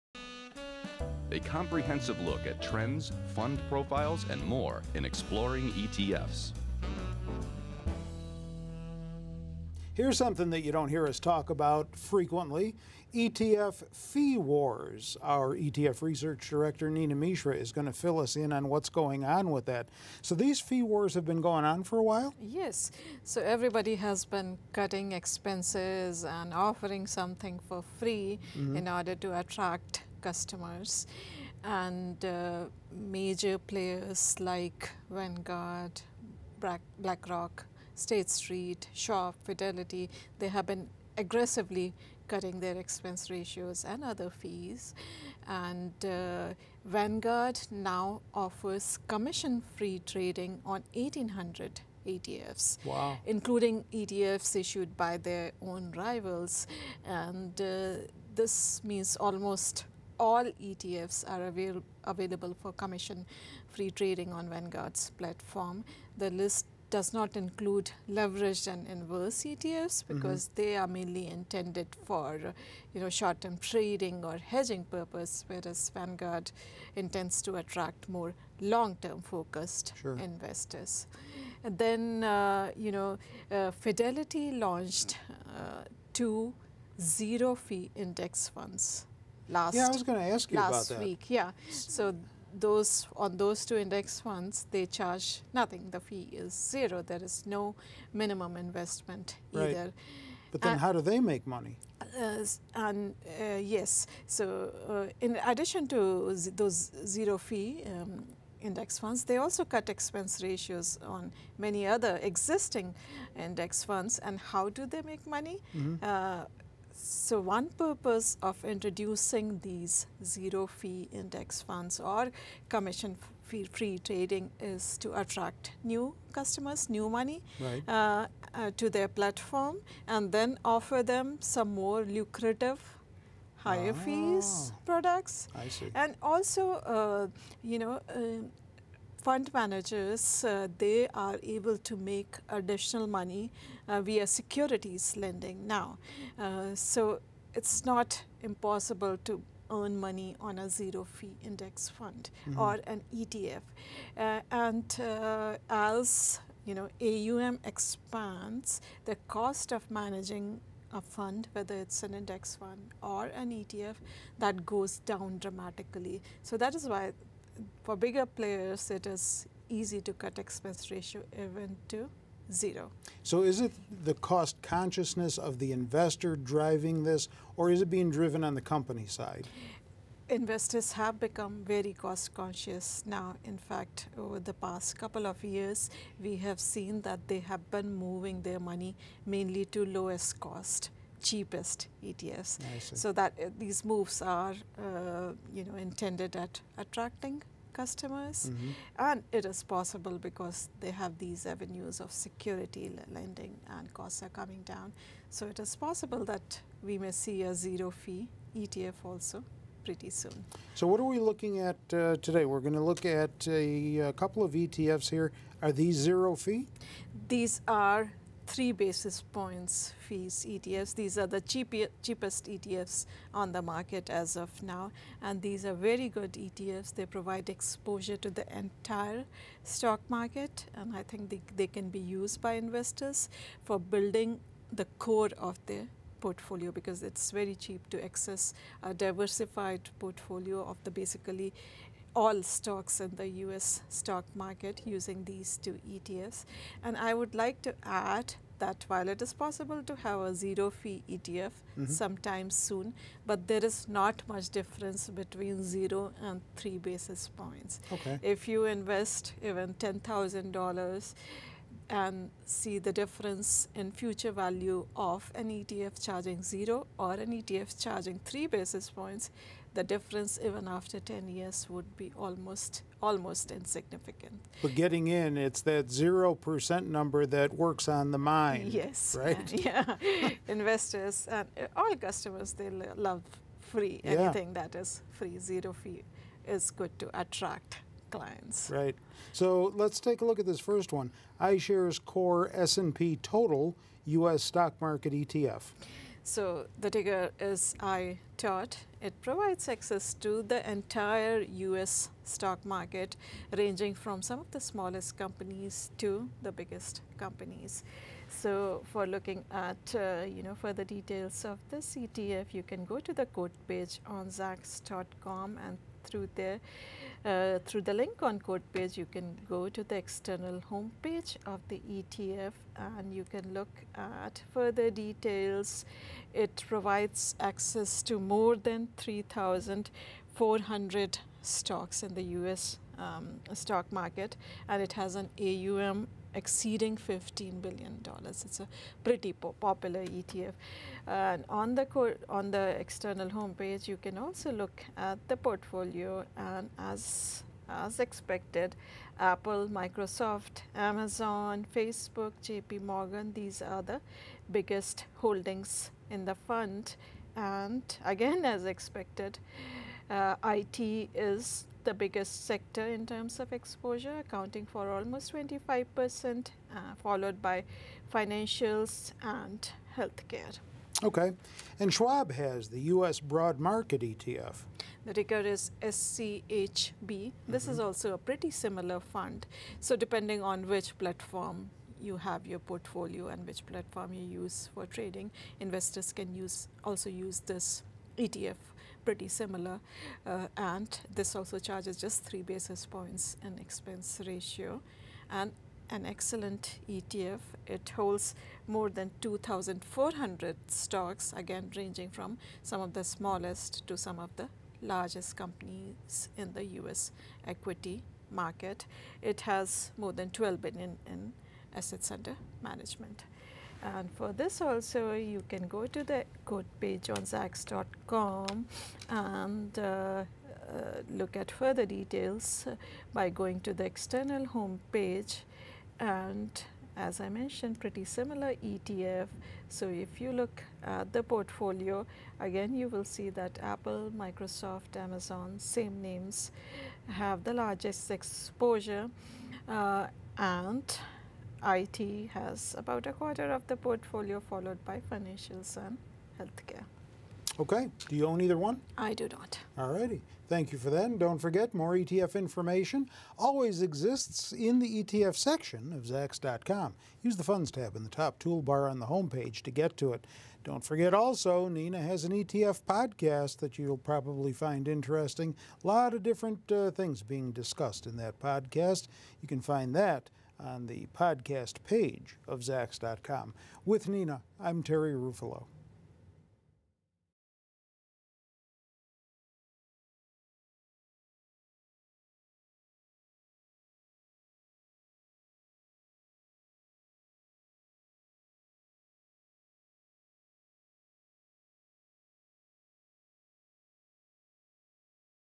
Cheapest ETFs in Focus as Fee War Heats Up
We discuss how ETF price war is a boon for investors and highlight two cheapest ETFs that provide diversified exposure to the entire US equity market.